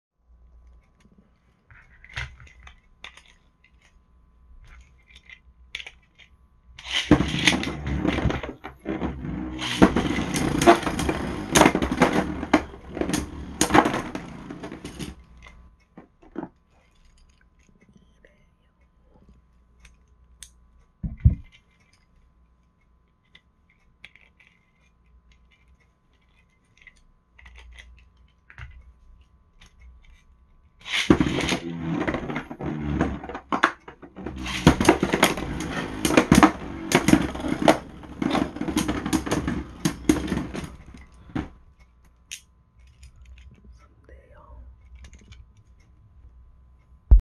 [EXHIBITION - BBB] 슈퍼킹 VS 갓레이어시스템 레이지롱기누스 Ds'.3A VS 지크엑스칼리버 1D.Ir 베이블레이드버스트
Apink핑크석기의 장난감팽이 베이블레이드버스트
핑크석기의 베이배틀!!
베이블레이드버스트슈퍼킹 시즌5베이 슈퍼킹레이어시스템 레이지롱기누스 Ds'.3A

레이지롱기누스 Ds'.3A(디스트로이대시 쓰리 어택)
지크엑스칼리버 1D.Ir(원 대거 아이언)

핑크석기 YouTube 구독